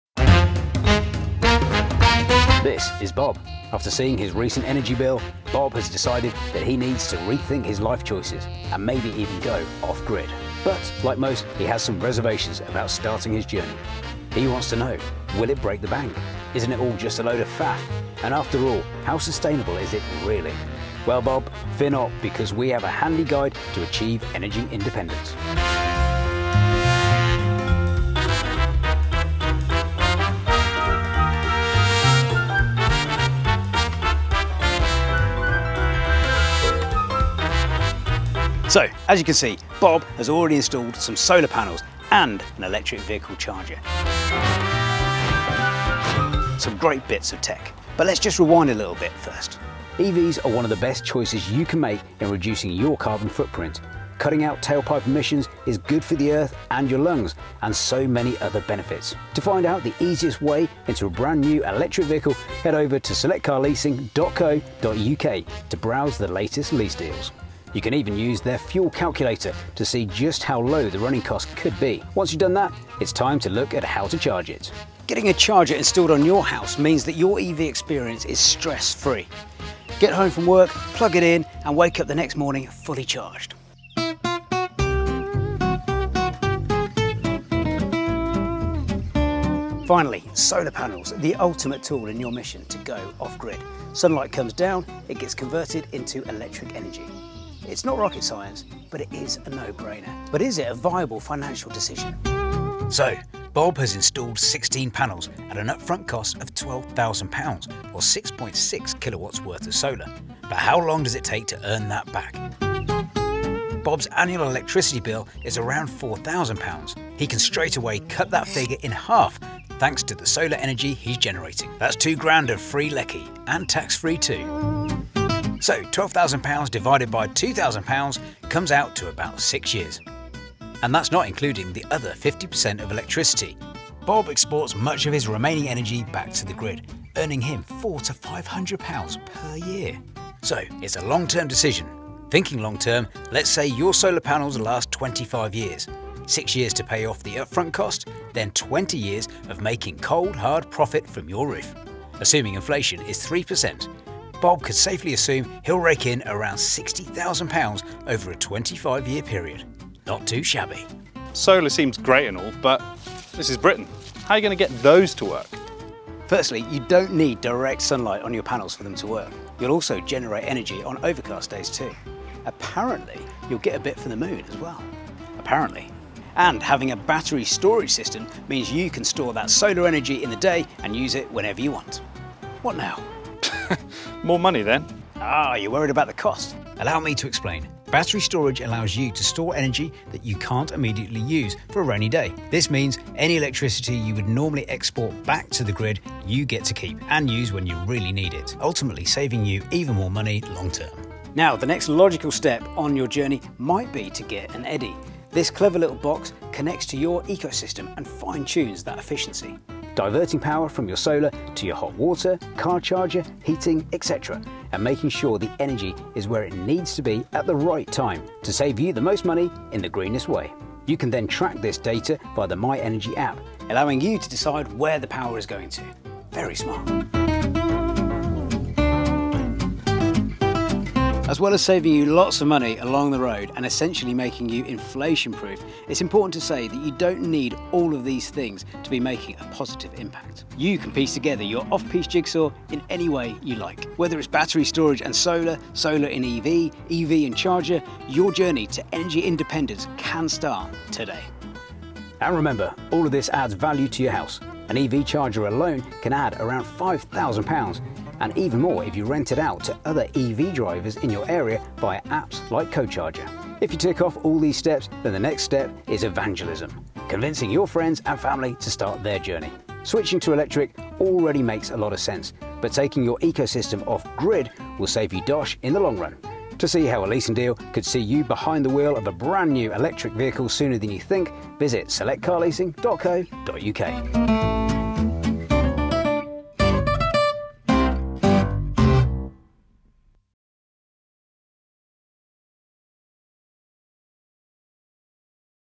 Select Explains: Going Off Grid
Looking to achieve energy independence? No worries...

Here's a handy guide to put your worries aside when considering going off grid.

Expensive? Faff? Sustainable? All questions answered in this video.

00:00:00 - Intro
00:00:30 - Overview
00:01:11 - How do you charge your EV?
00:01:25 - Solar Panels
00:03:21 - Become more energy efficient with an Eddi
00:03:52 - Other ways to save money and gain energy independence

To find out everything you need to know about electric vehicles and going off grid,